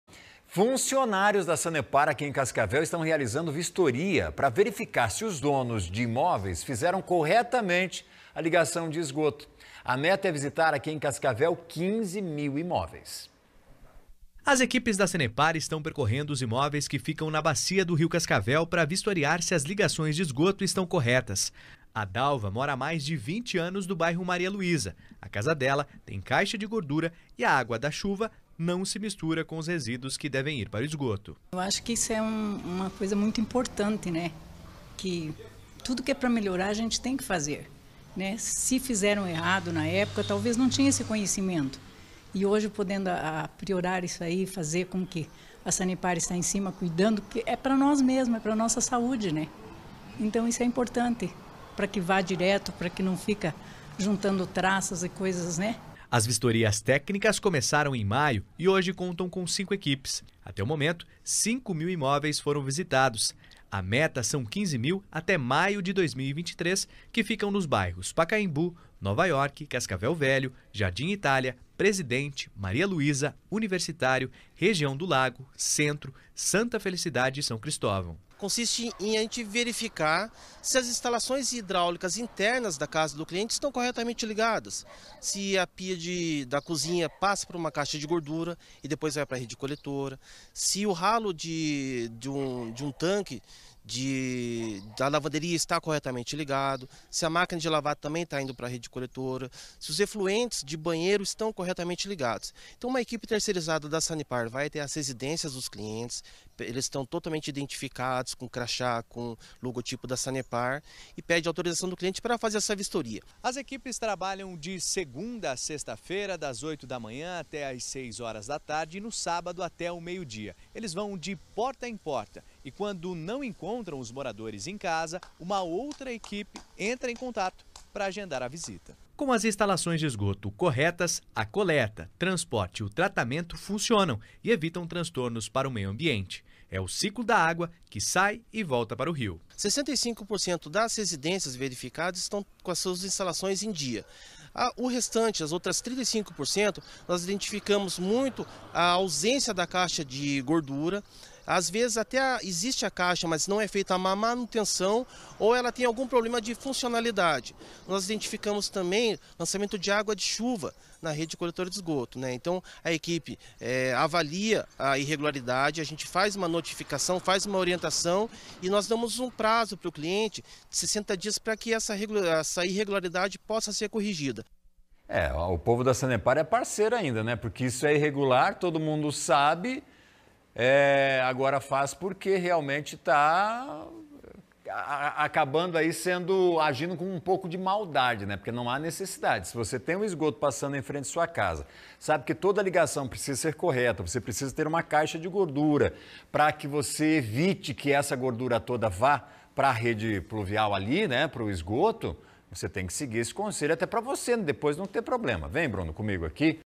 Sanepar faz vistoria para verificar se donos de imóveis conectaram esgoto corretamente
Funcionários da Sanepar estão realizando vistorias, para verificar se os donos de imóveis fizeram a ligação do esgoto corretamente. A meta é visitar 15 mil imóveis em Cascavel.